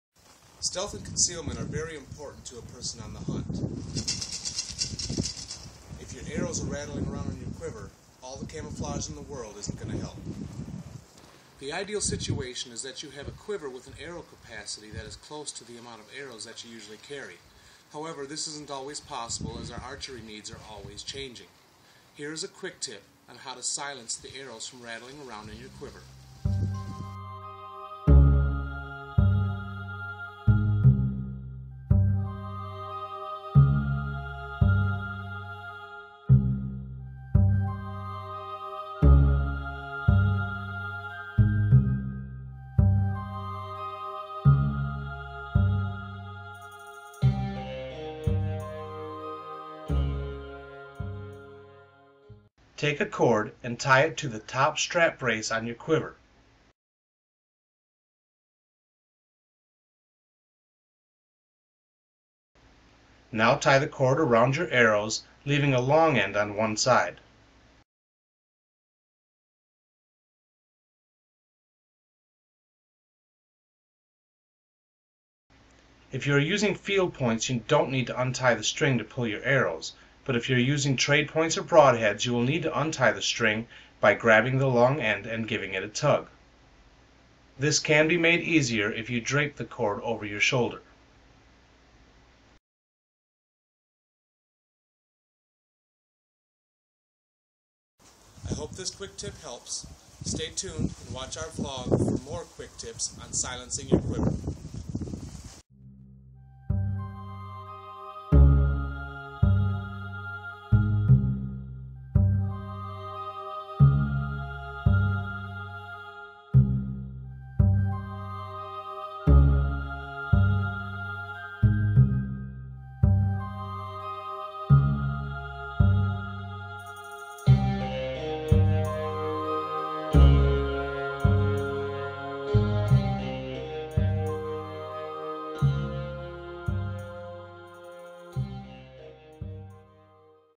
Rasher Quivers - Silencing Your Quiver (Vlog #3)
Jason offers a quick tip on how to limit the noise from your quiver caused by rattling arrows.